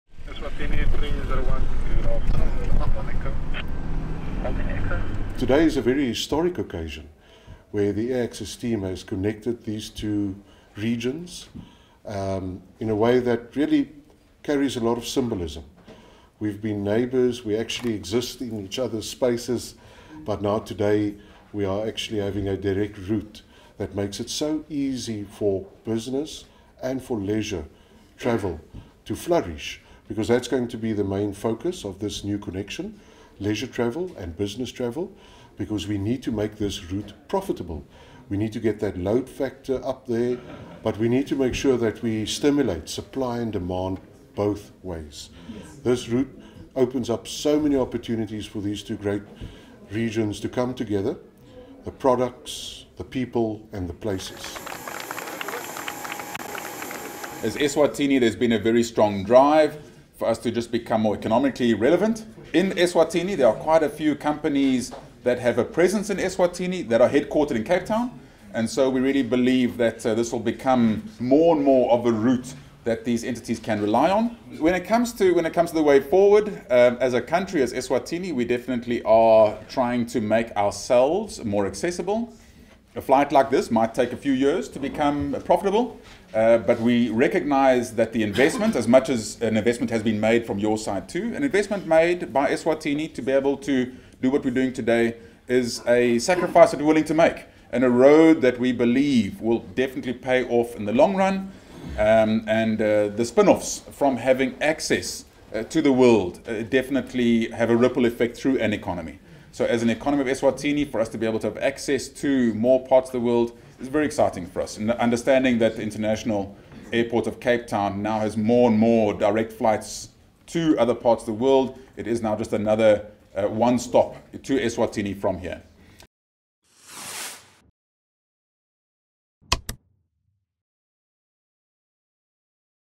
WATCH | Eswatini Air touches down in Cape Town, offering three flights a week in the city
Recently launched Eswatini Air has added Cape Town to its list of South African destinations after already landing in Johannesburg and Durban earlier this year.